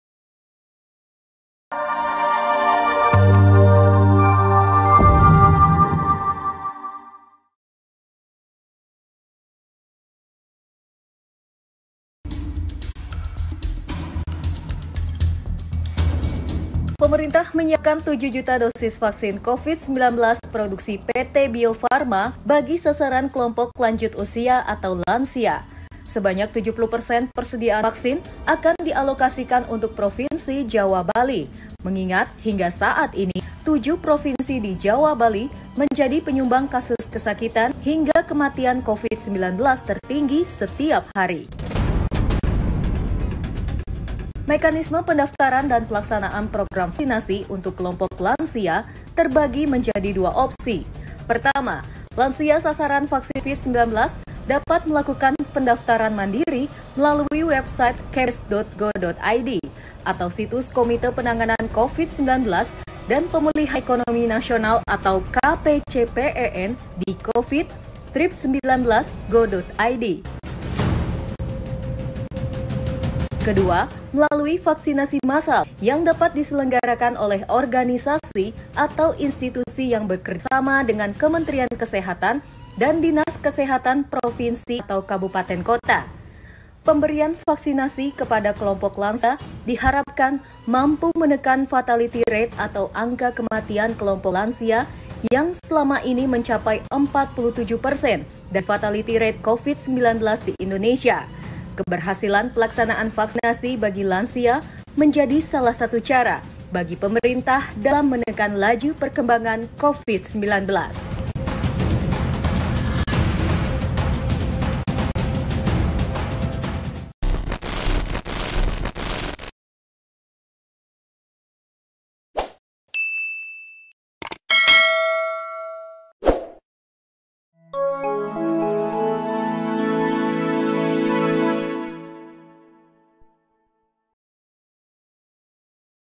MEKANISME VAKSIN UNTUK LANSIA
Lansia menjadi kelompok prioritas untuk divaksin. Karena kelompok ini dinilai yang paling banyak mendorong angka kematian akibat virus corona. Hal itu terlihat dari anga fatility rate yang paling banyak dikontribusikan dari kelompok lansia.